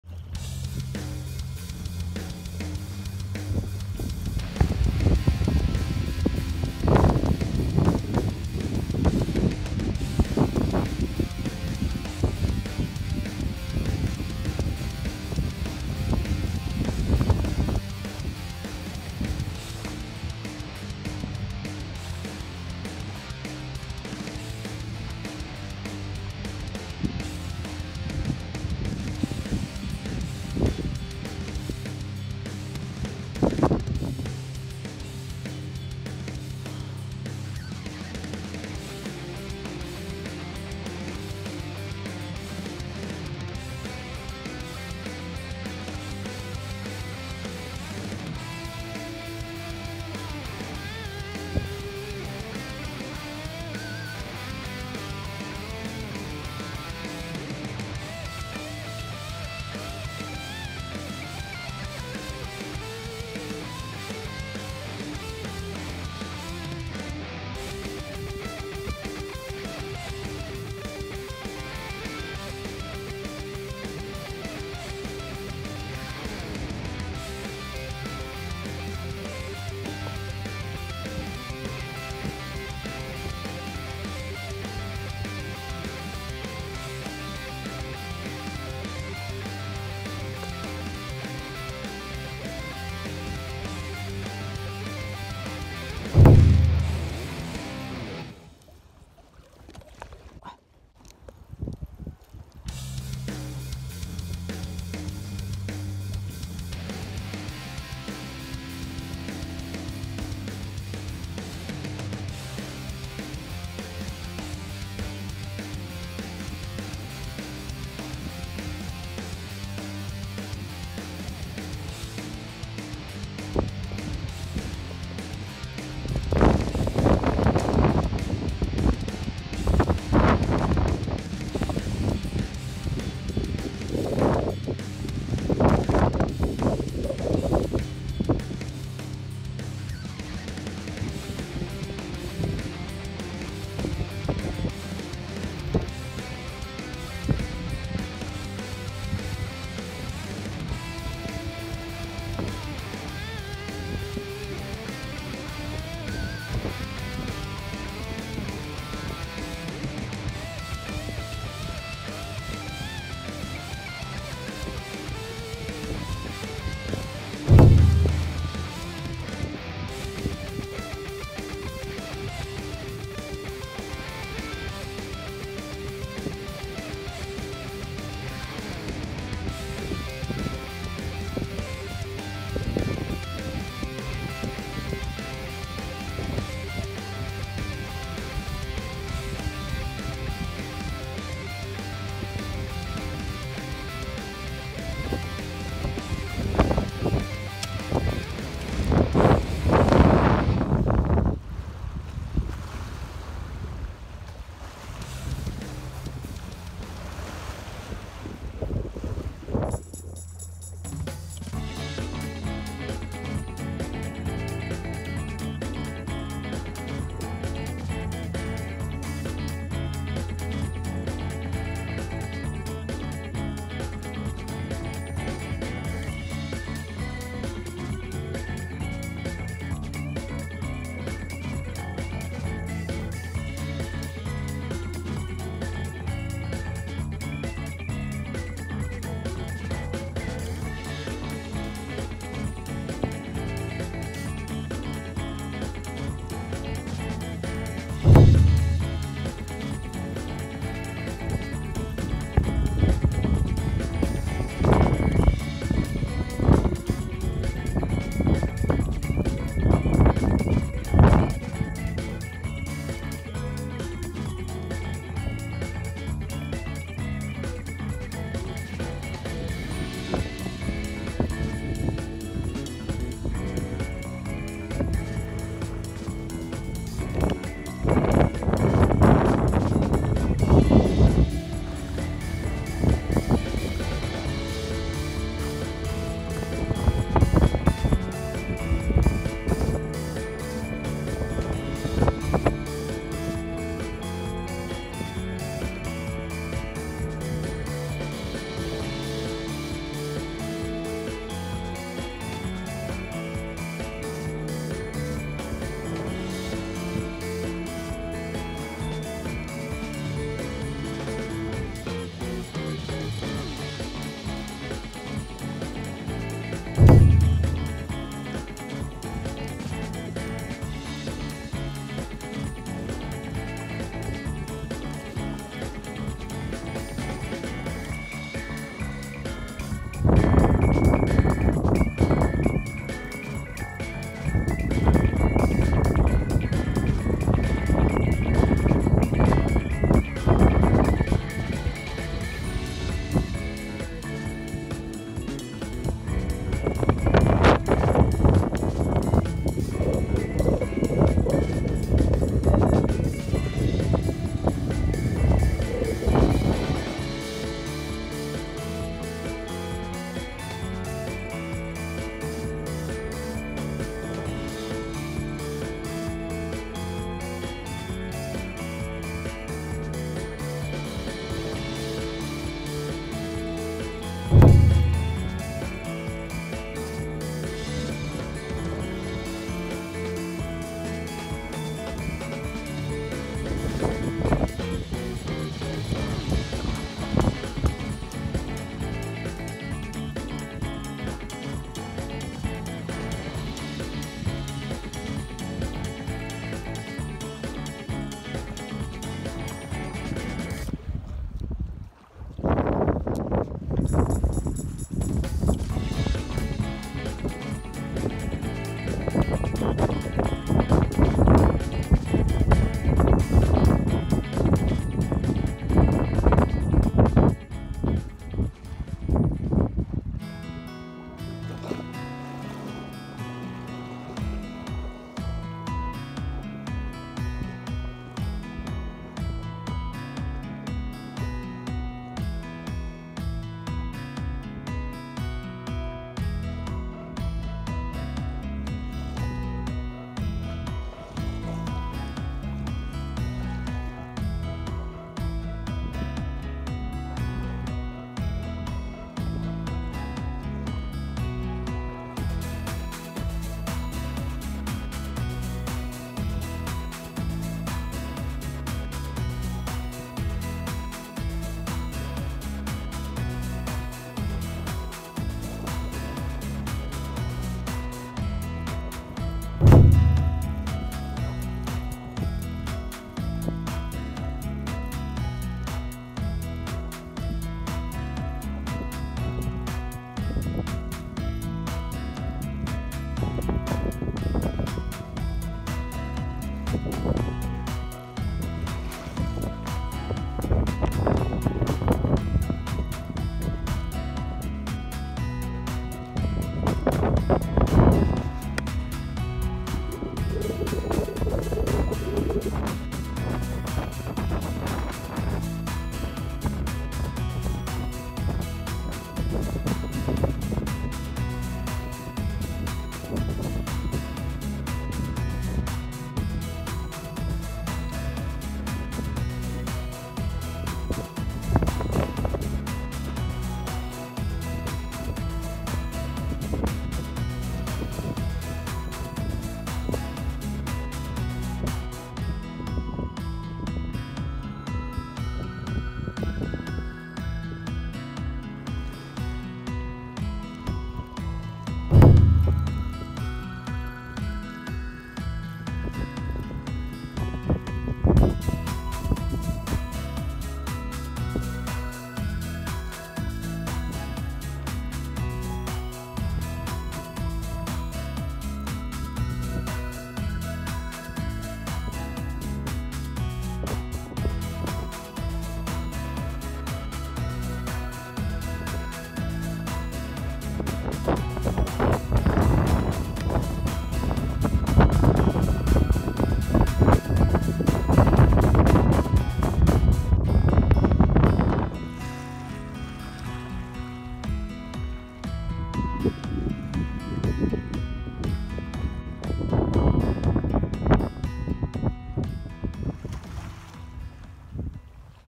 11月のキス釣りは？キスはいつまで釣れる？知多半島
11月のキス釣りは？キスはいつまで釣れる？知多半島です。
シロギス、知多湾、南知多、投げ釣り、ちょい投げ

□使用BGM

・魔王魂